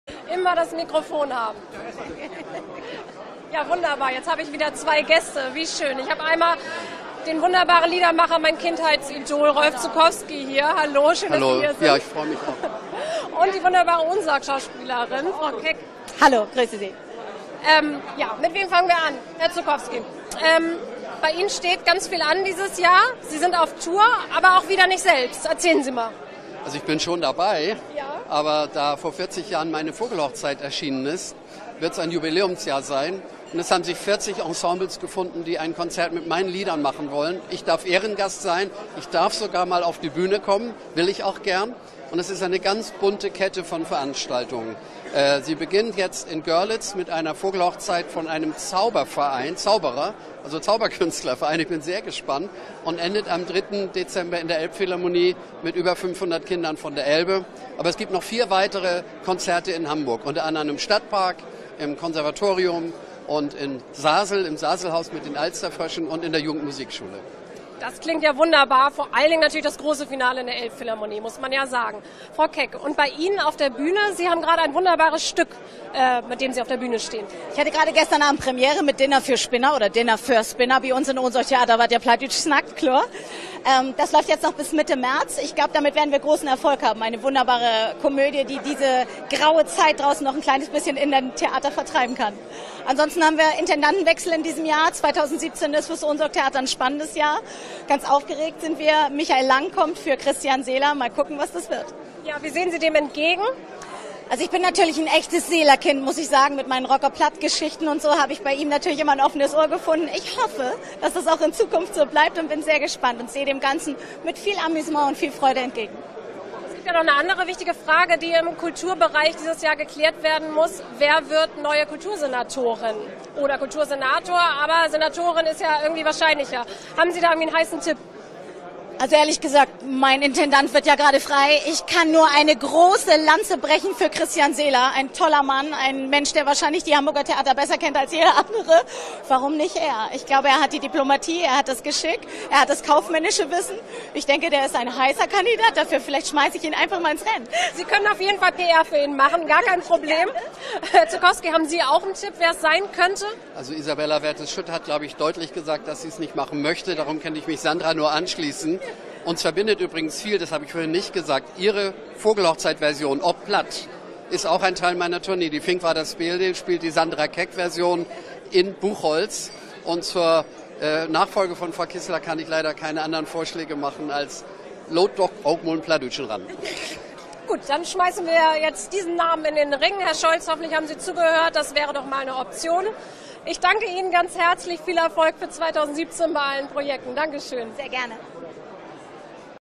Sandra Keck (Schauspielerin), Rolf Zuckowski (Liedermacher) Hamburger Abendblatt Neujahrsempfang2017
Der Empfang mit fast 1000 Gästen aus Politik, Wirtschaft, Sport und Kultur im Hotel Atlantic.